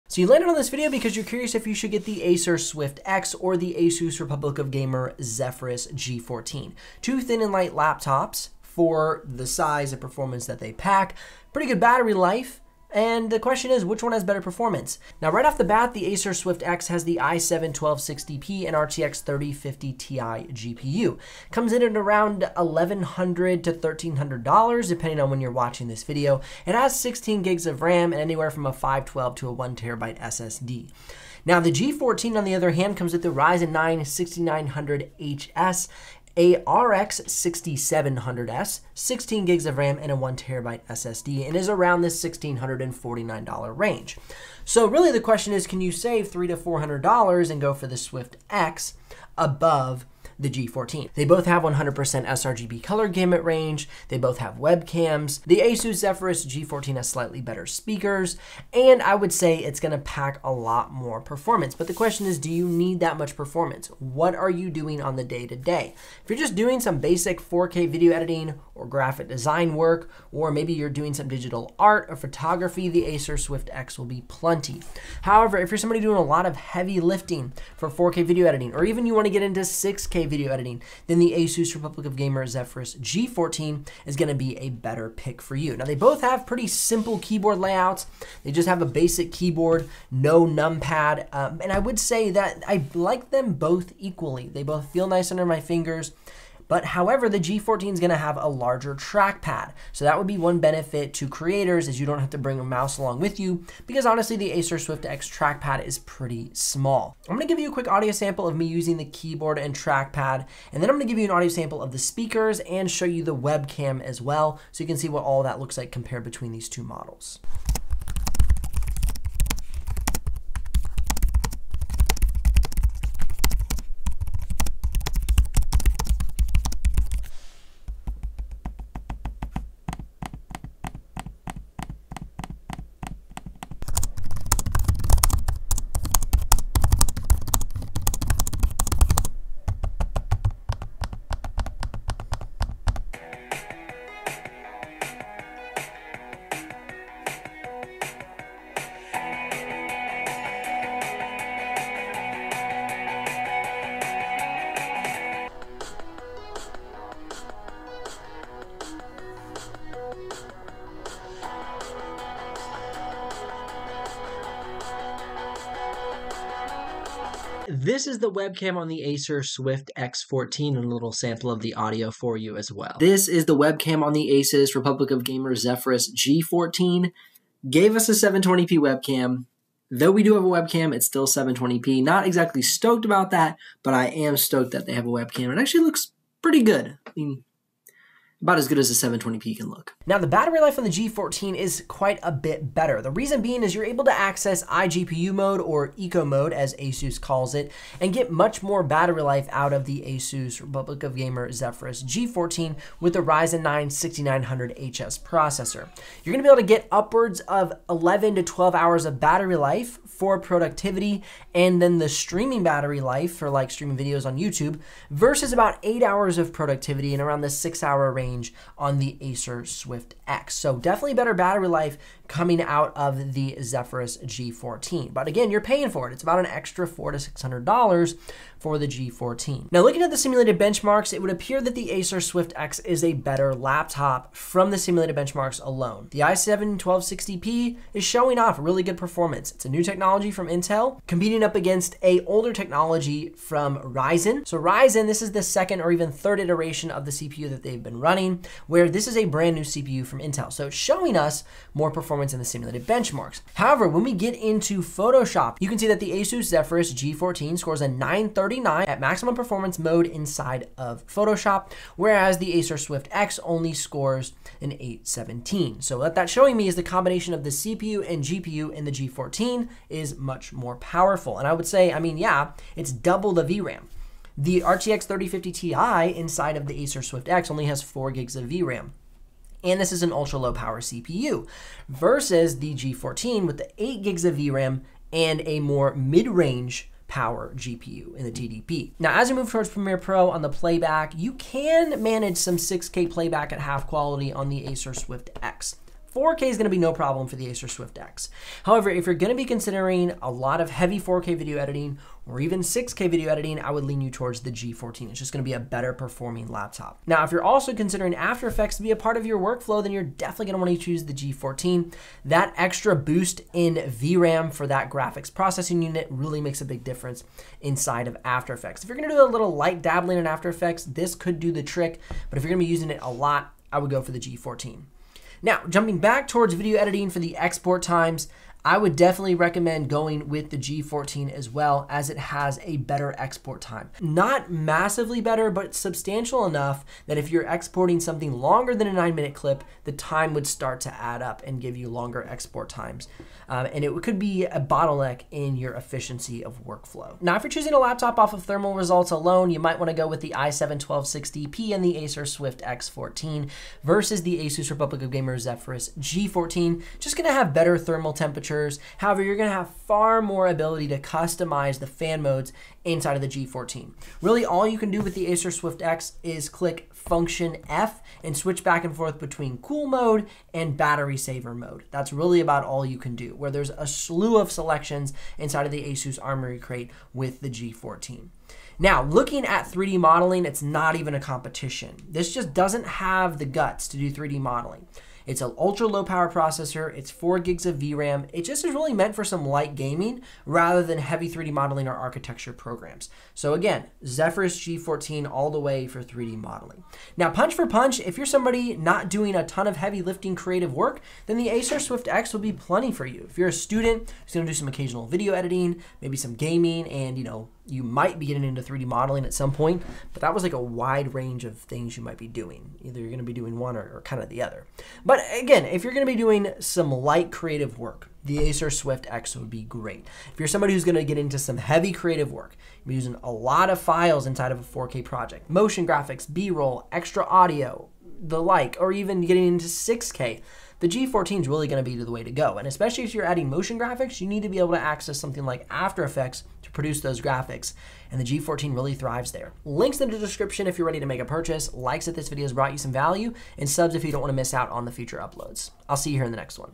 Don't try to save $600 | Acer Swift X 14 vs Asus ROG Zephyrus G14
In this video, I'm reviewing the Acer Swift X 14 Vs Asus ROG Zephyrus G14. These 14-inch laptops are both on-the-go friendly but one performs better and has MUCH better battery life.

💻 Acer Swift X 14 - Commission Earned )
💻 Asus ROG Zephyrus G14 RX6700s - Commission Earned )
💻 Asus ROG Zephyrus G14 RX6800s - Commission Earned )

If you're looking for a 14-inch gaming laptop as a creator then I would hands-down choose the Asus ROG Zephyrus G14 over the Acer Swift X 14. Now, you could save around $600 by choosing the Acer Swift X, but to me when you look at the performance and the battery life on the Asus Zephyrus G14 it is not even a competition.

0:00 - Acer Swift X vs. Asus Zephyrus Intro
0:15 - Specs
1:24 - Keyboard & Trackpad
1:57 - Keyboard & Trackpad Audio Sample
2:22 - Speaker Audio Sample
2:47 - Webcams
3:09 - Battery Life
3:49 - CineBench R23 Single Core & Multi-Core
3:59 - Geekbench Single Core & Multi-Core
4:13 - Photoshop Benchmark
4:48 - Dropped Frames
5:06 - After Effects Benchmark
5:27 - Export Times
5:46 - Thermals
6:18 - Autodesk Maya, Autodesk 3DS Max, PTC Creo, & Solidworks
6:37 - Final Thoughts